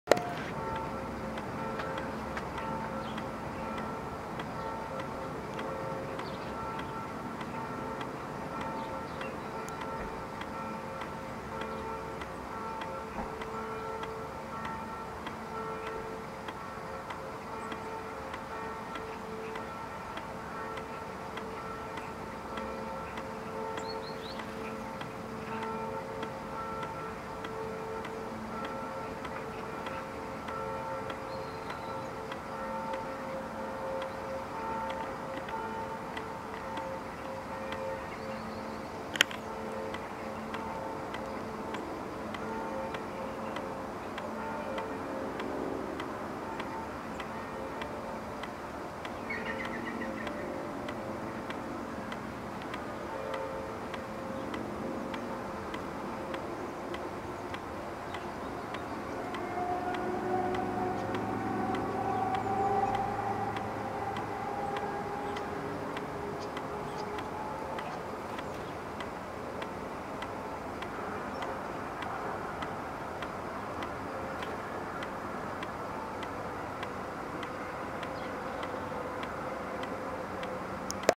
Blue tit eats its peanuts
a green woodpecker passes by - sound only
September 2019